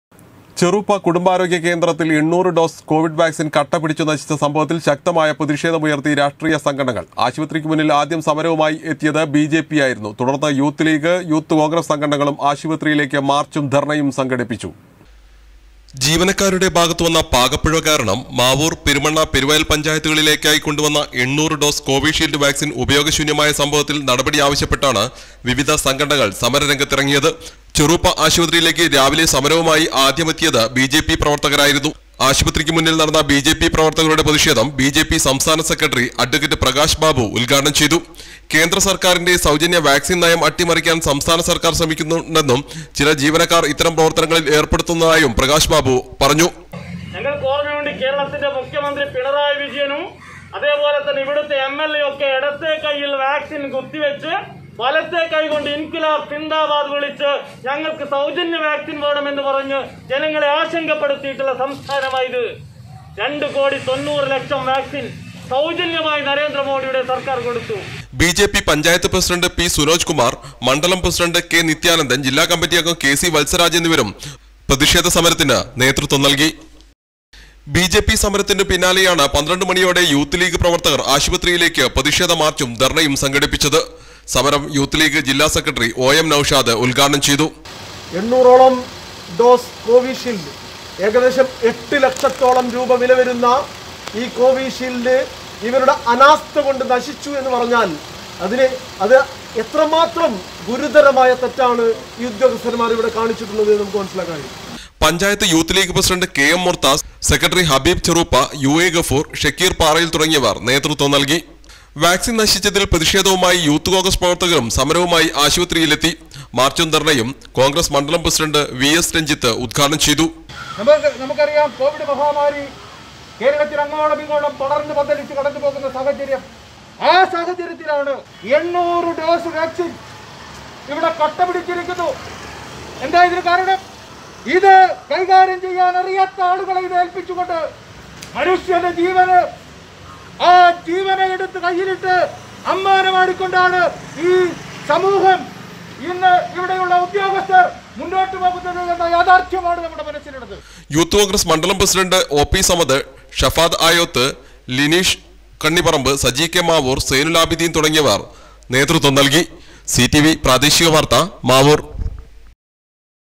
800 ഡോസ് കോവിഡ് വാക്സിൻ കട്ടപിടിച്ചു നശിച്ച സംഭവത്തിൽ ശക്തമായ പ്രതിഷേധം ഉയർത്തി
ചെറുപ്പ കുടുംബ ആരോഗ്യ കേന്ദ്രത്തിൽ 800 ഡോസ് കോവിഡ് വാക്സിൻ  കട്ടപിടിച്ചു നശിച്ച സംഭവത്തിൽ ശക്തമായ പ്രതിഷേധം ഉയർത്തി രാഷ്ട്രീയ സംഘടനകൾ ആശുപത്രിക്കുമുന്നിൽ ആദ്യം സമരവുമായി എത്തിയത് ബിജെപി ആയിരുന്നു  .തുടർന്ന് യൂത്ത് ലീഗ്
യൂത്ത് കോൺഗ്രസ് സംഘടനകളും  ആശുപത്രിയിലേക്ക് മാർച്ചും ധർണ്ണയും സംഘടിപ്പിച്ചു